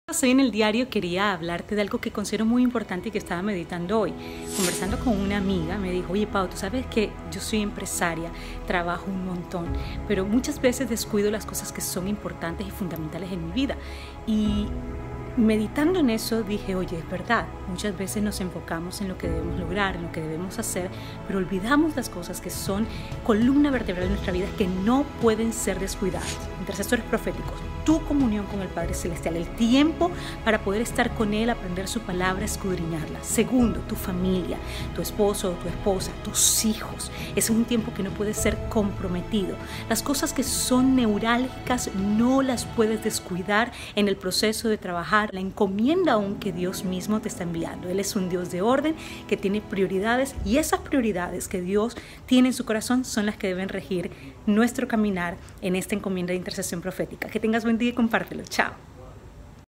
Las Prioridades de un Intercesor
diariodeunintercesorprofetico En el cumplimiento de tu encomienda no descuides aquello que es prioritario: tu intimidad con el Padre, tu familia, el estudio de las Escrituras, tu salud, en fin, todo aquello que debe ser honrado, valorado y preservado. In the fulfillment of your commission do not neglect that which is a priority: your intimacy with the Father, your family, the study of the Scriptures, your health, in short, everything that should be honored, valued and preserved. vozprofetica familia ministerio honra @paolasantamariadeporras @ministerioredil